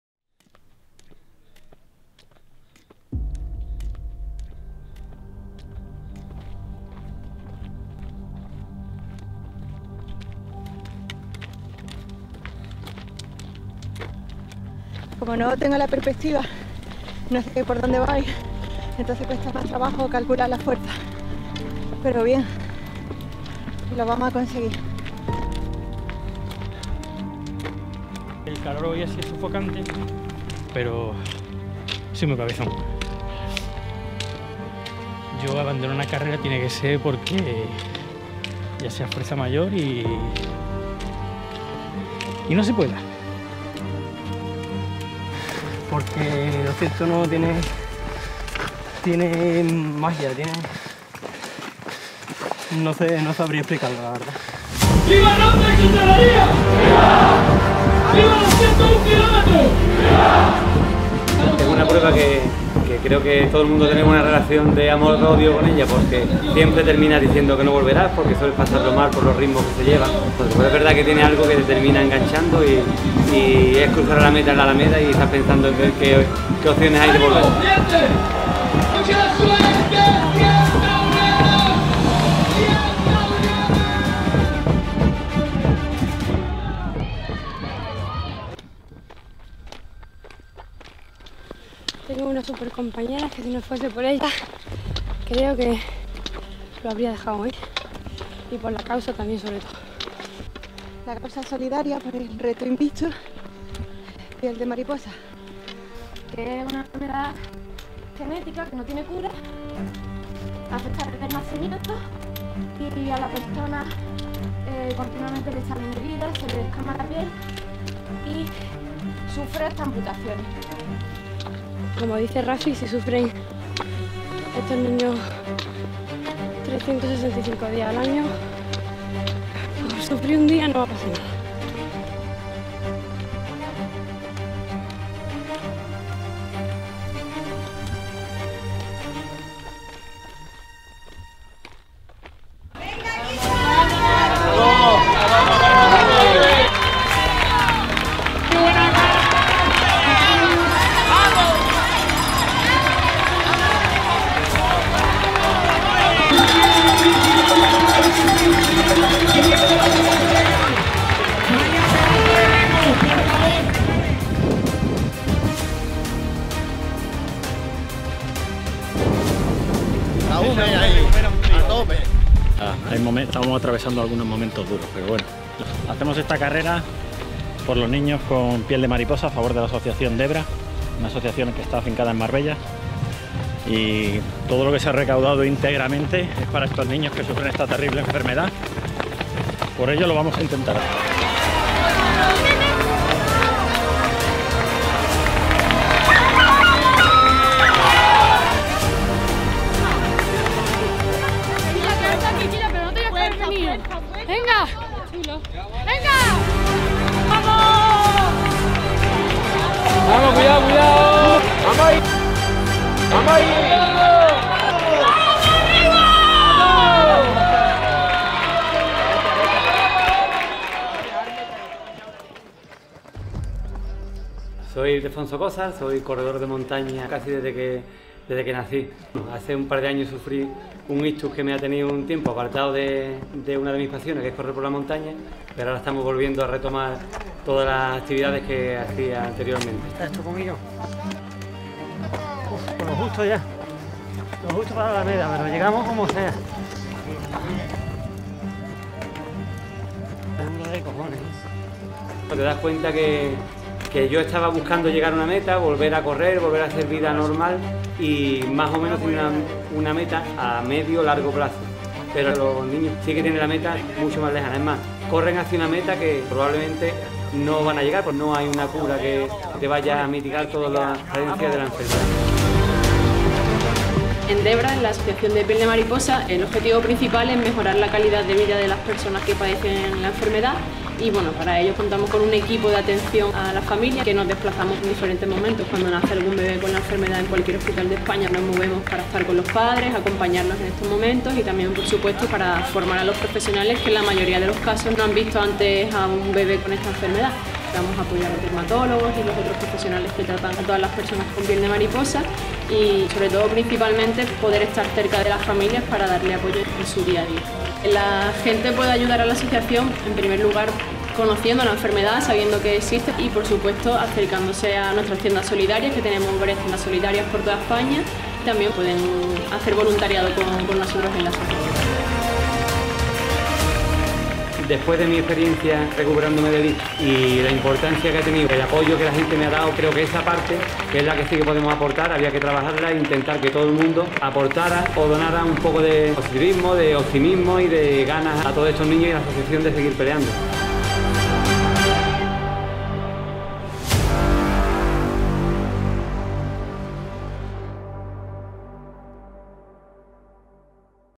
CREDO - Minidocumental 101km de Ronda con la Piel de Mariposa
CREDO es un minidocumental grabado en los 101km de Ronda de 2019, junto a los equipos INVICTUS y MARIPOSAS INVICTUS, que hicieron la prueba por la enfermedad conocida como Piel de Mariposa.